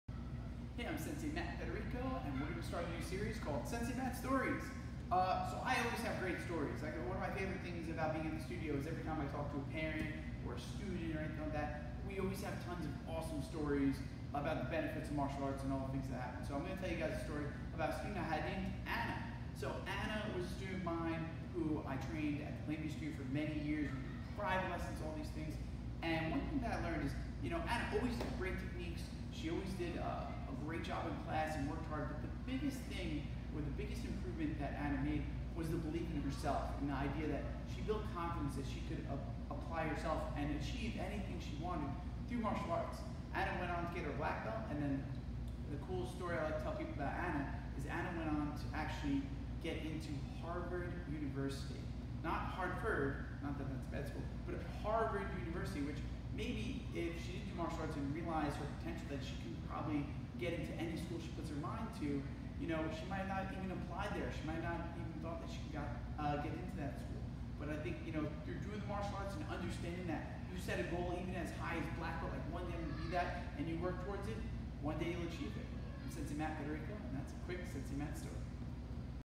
How Karate Got Me Into Harvard - Ana's Story
This is a story about one of our students, Ana, who was able to use what she learned in karate to get into Harvard University!